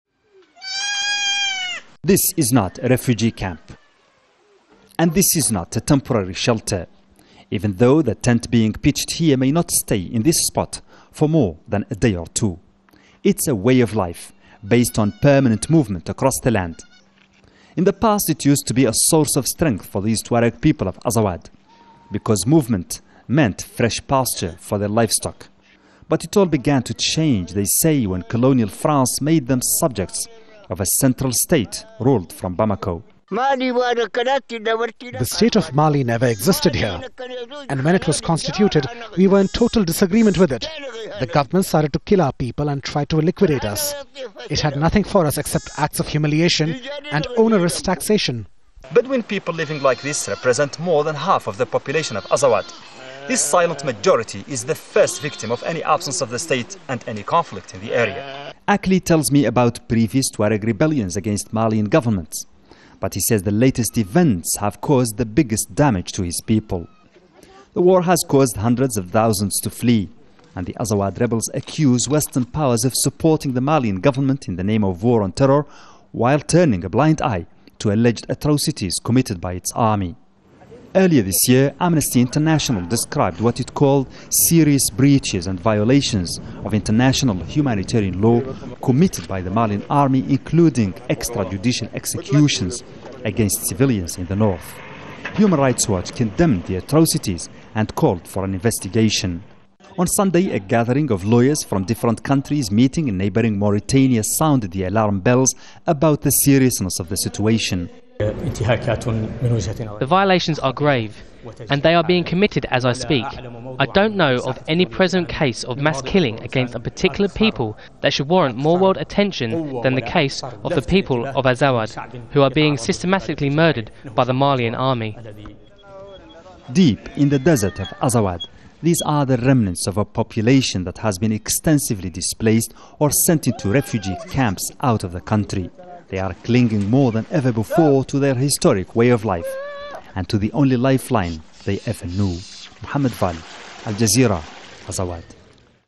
Mali nomads accuse army of atrocities
Thousands of Tuareg bedouins have fled the north of Mali during months of conflict.
Rebels declared independence in the region of Azawad last year. But since January, French and Malian troops have been trying to drive them out.

The Azawad rebels have accused Western powers of supporting the government in Bamako while turning a blind eye to alleged atrocities committed by its army.
Al Jazeera's Mohamed Vall reports from Azawad.